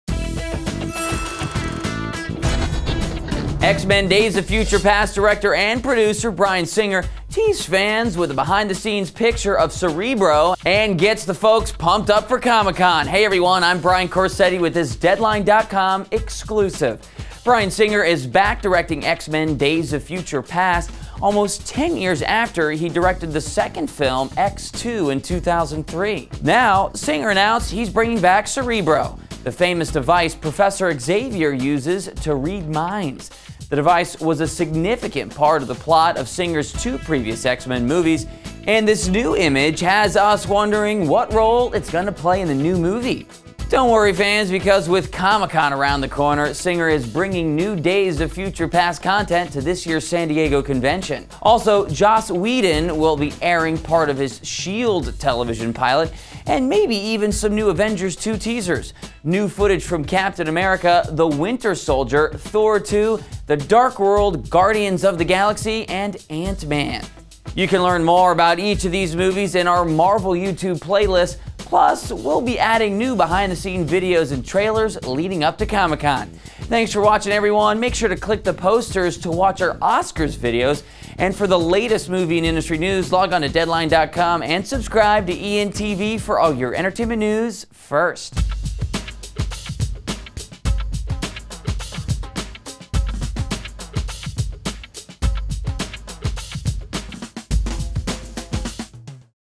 Marvel 2013 Comic Con Preview : Days of Future Past, S.H.I.E.L.D., Captain America 2 - ENTV
We've got you Marvel Comic Con 2013 Preview! This is a huge year for Marvel Movies/TV. Joss Whedon's S.H.I.E.L.D. will be headlining with pilot footage, X-Men Days of Future Past, Avengers 2 and more. Let us know which movie/footage you want to see the most! New Update: Matthew Vaughn former Days of Future Past director is now producing the Fantastic Four reboot.

Marvel Movies at Comic Con 2013:
S.H.I.E.L.D. (TV)
Thor 2 - The Dark World
Ant Man
Guardians of The Galaxy
Avengers 2 (rumored)